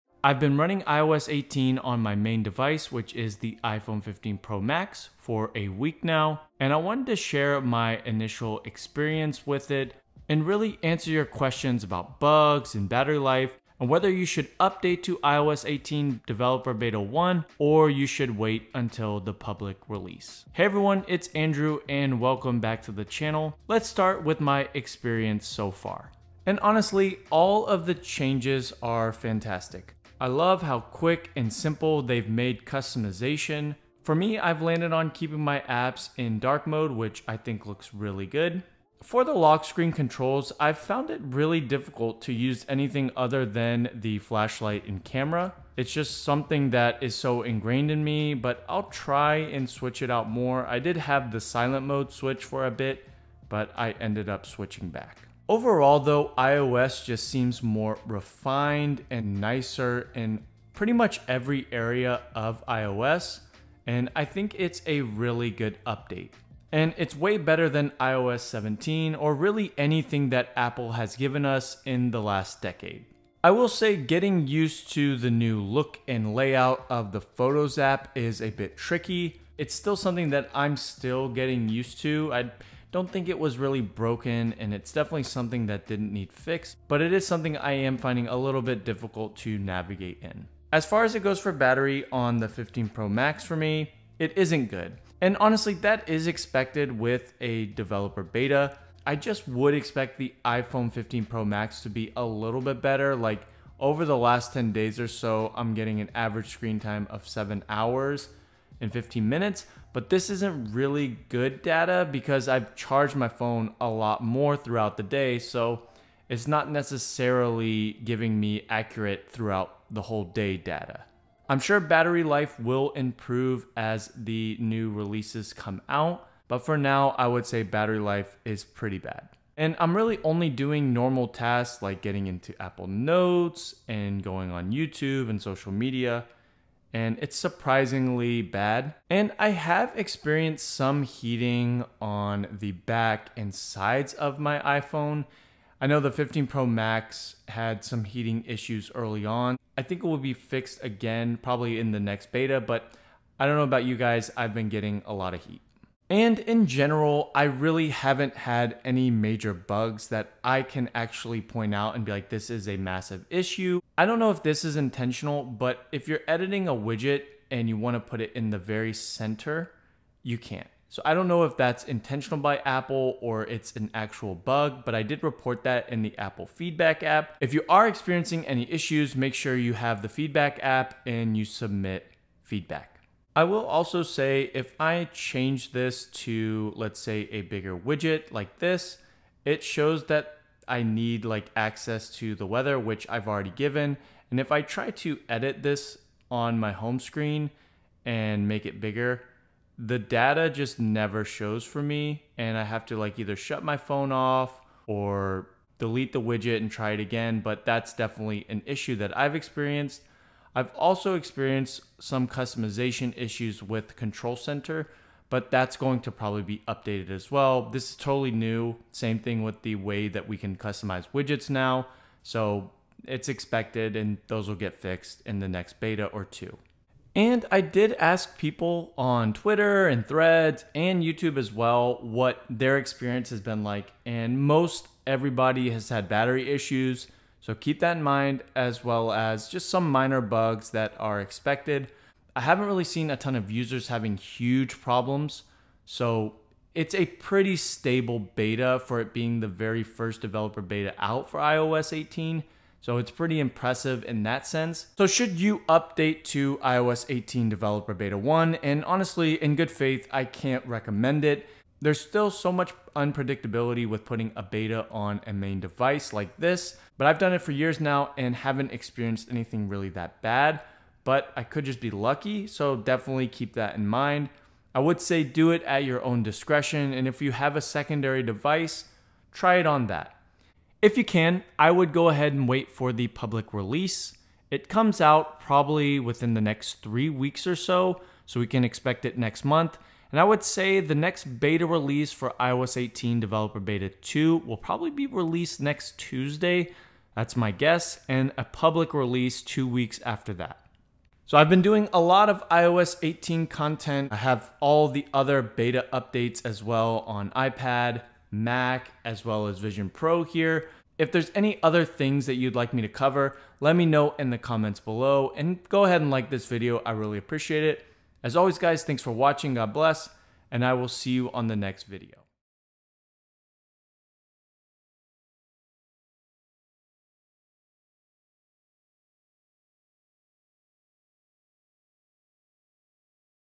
iOS 18 Beta 1 - Bugs, Battery Life & Issues on iPhone 15 Pro Max!
Product links:

Alternate Keyboard
Headphones stand
Docking Station
Wire management
iPad Mount